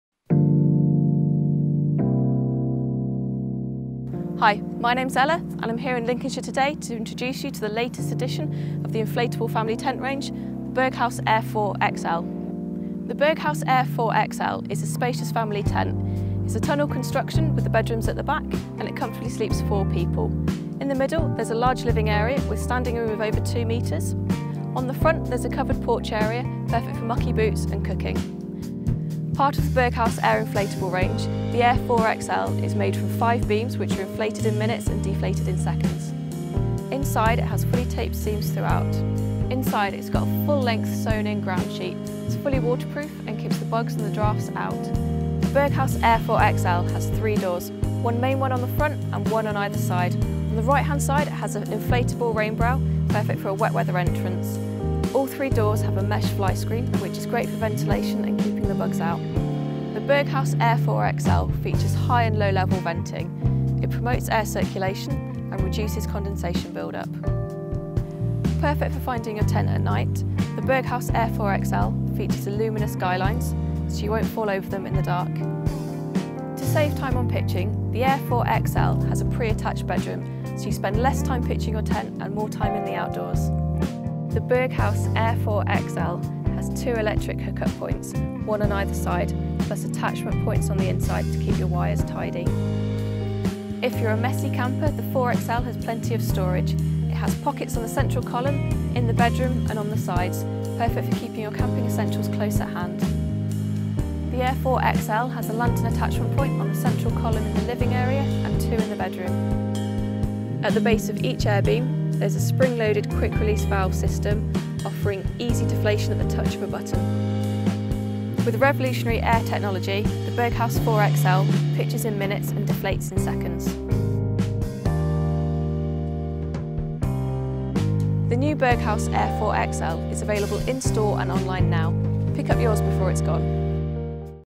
Berghaus Air 4 XL Tent | Product Review
Customisable, comfortable and convenient, the Berghaus Air 4 XL is a quick-fire home away from home. Easily erected and deflated, using inflatable poles and quick release valves, the Air 4 XL also offers the versatility to suit your camping group. Removable interior walls give you the choice of single/multiple bedrooms or an open-plan living space. Mesh bedroom doors and adjustable curtains allow you to balance light levels with privacy so that you can build the space you want, wherever you are.